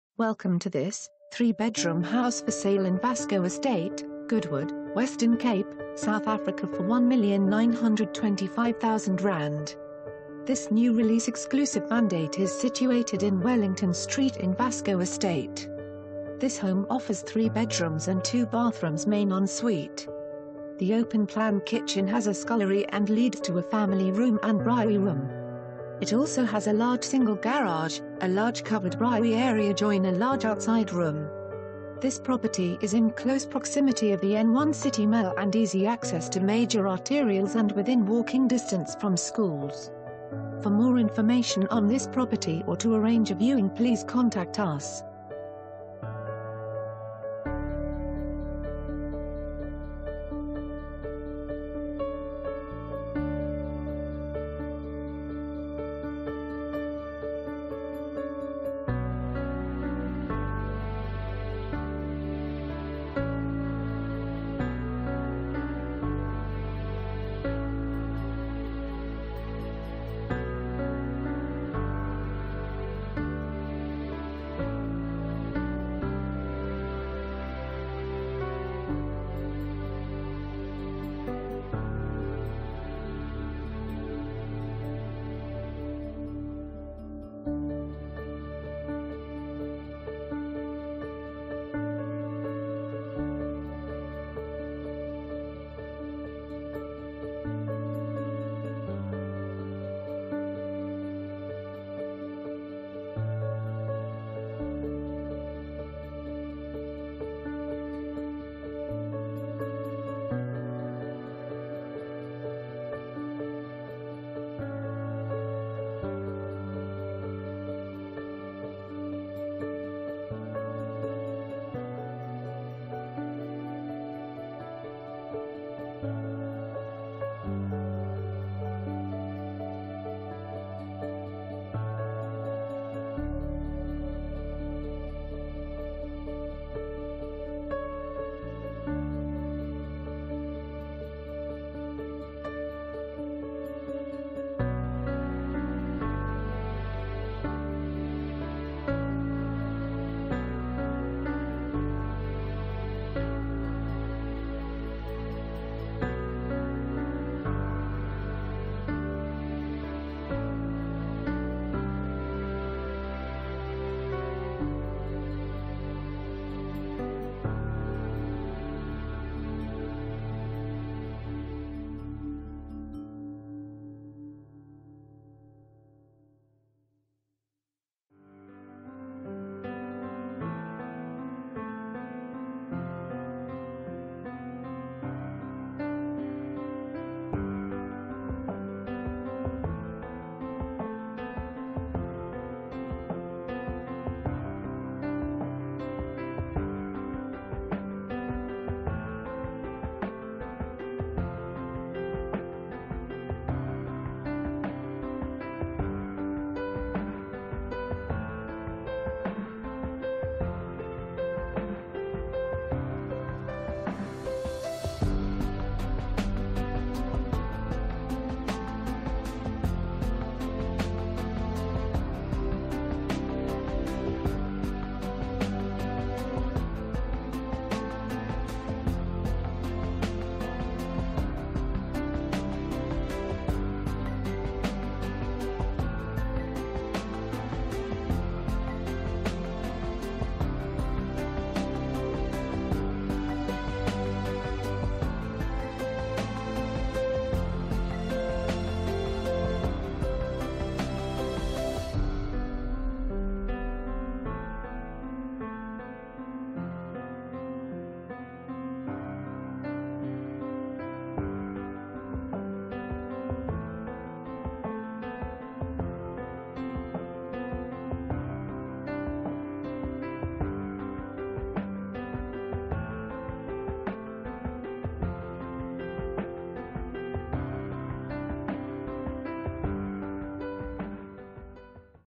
3 Bedroom House For Sale in Vasco Estate, Goodwood, Western Cape, South Africa for ZAR 1,925,000
This new release (exclusive mandate) is situated in Wellington Street in Vasco Estate.

This home offers three bedrooms and two bathrooms (Main en-suite). The open plan kitchen has a scullery and leads to a family room and braai room. It also has a large single garage, a large covered braai area join a large outside room.

Th...